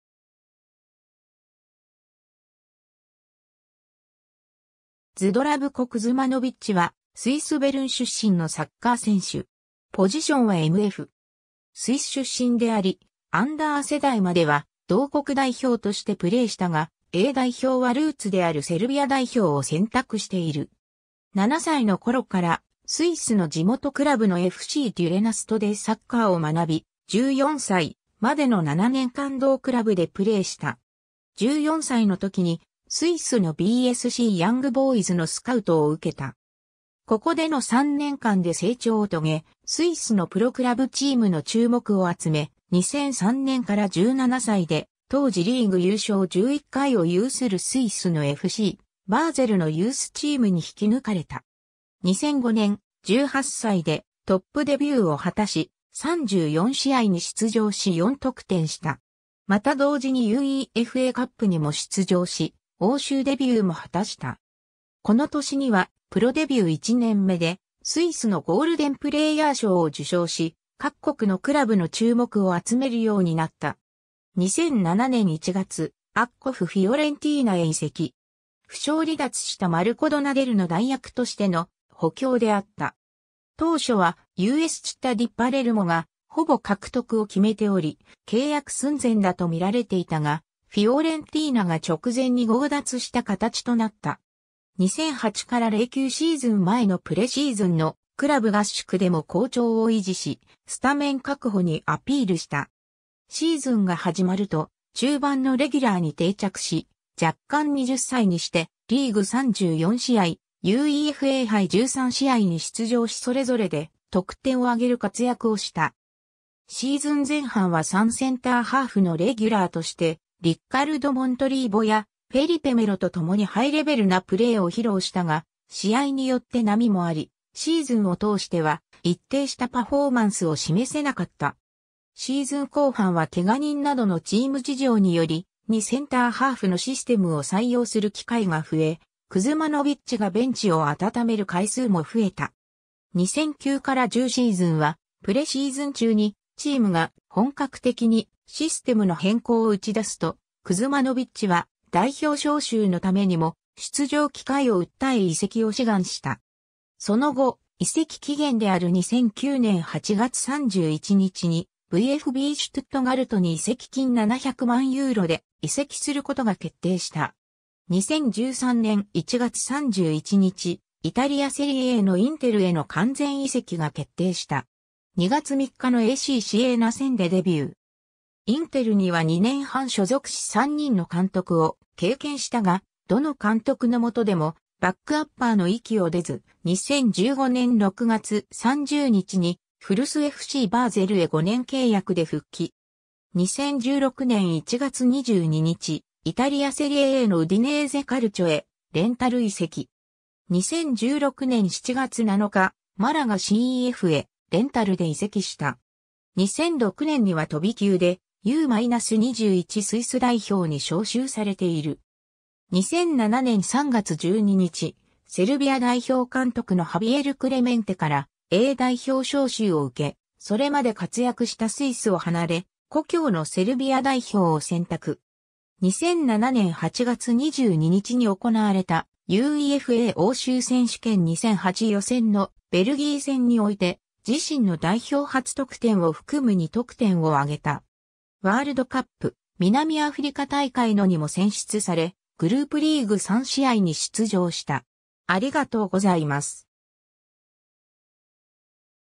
ズドラヴコ・クズマノヴィッチ
ズドラヴコ・クズマノヴィッチ（Здравко Кузмановић, Zdravko Kuzmanović, 1987年9月22日 - ）は、スイス・ベルン出身のサッカー選手。
 スイス出身であり、アンダー世代までは同国代表としてプレーしたが、A代表はルーツであるセルビア代表を選択している。
 7歳の頃からスイスの地元クラブのFCデュレナストでサッカーを学び、14歳までの7年間同クラブでプレーした。
 14歳の時に、スイスのBSCヤングボーイズのスカウトを受けた。
ここでの3年間で成長を遂げ、スイスのプロクラブチームの注目を集め、2003年から17歳で、当時リーグ優勝11回を有するスイスのFCバーゼルのユースチームに引き抜かれた。
 2005年、18歳でトップデビューを果たし、34試合に出場し4得点した。
また同時にUEFAカップにも出場し欧州デビューも果たした。
この年にはプロデビュー1年目でスイスのゴールデンプレーヤー賞を受賞し、各国のクラブの注目を集めるようになった。
 2007年1月、ACFフィオレンティーナへ移籍。
負傷離脱したマルコ・ドナデルの代役としての補強であった。
当初はUSチッタ・ディ・パレルモがほぼ獲得を決めており契約寸前だと見られていたが、フィオレンティーナが直前に強奪した形となった。
 2008-09シーズン前のプレシーズンのクラブ合宿でも好調を維持し、スタメン確保にアピールした。
シーズンが始まると中盤のレギュラーに定着し、弱冠20歳にしてリーグ34試合、UEFA杯13試合に出場しそれぞれで得点を挙げる活躍をした。
シーズン前半は3センターハーフのレギュラーとしてリッカルド・モントリーヴォやフェリペ・メロと共にハイレベルなプレーを披露したが、試合によって波もありシーズンを通しては一定したパフォーマンスを示せなかった。
シーズン後半は怪我人などのチーム事情により、2センターハーフのシステムを採用する機会が増え、クズマノヴィッチがベンチを暖める回数も増えた。
2009-10シーズンは、プレシーズン中にチームが本格的にシステムの変更を打ち出すと、クズマノヴィッチは代表招集のためにも出場機会を訴え移籍を志願した。
その後、移籍期限である2009年8月31日にVfBシュトゥットガルトに移籍金700万ユーロで移籍することが決定した。
 2013年1月31日、イタリア・セリエAのインテルへの完全移籍が決定した。
2月3日のACシエーナ戦でデビュー。
インテルには2年半所属し3人の監督を経験したが、どの監督の下でもバックアッパーの域を出ず、2015年6月30日に古巣FCバーゼルへ5年契約で復帰。
 2016年1月22日、イタリア・セリエAのウディネーゼ・カルチョへレンタル移籍。
2016年7月7日、マラガCFへレンタルで移籍した。
 2006年には飛び級でU-21スイス代表に招集されている。
 2007年3月12日、セルビア代表監督のハビエル・クレメンテからA代表招集を受け、それまで活躍したスイスを離れ、故郷のセルビア代表を選択。
2007年8月22日に行われたUEFA欧州選手権2008予選のベルギー戦において、自身の代表初得点を含む2得点を挙げた。
 ワールドカップ南アフリカ大会のにも選出され、グループリーグ3試合に出場した。